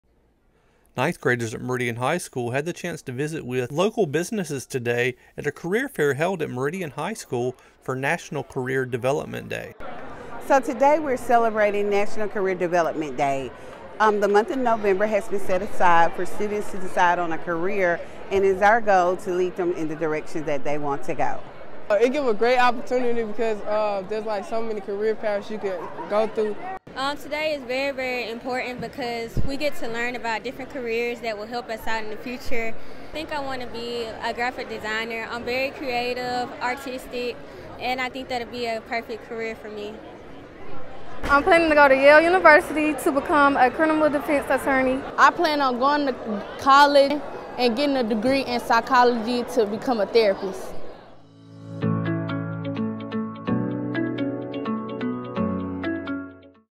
MHS 9th Grade Career Day
9th graders at Meridian High had the chance to explore career paths today during Career Day in honor of National Career Development Day! Thank you to all of our community partners who helped make this event possible!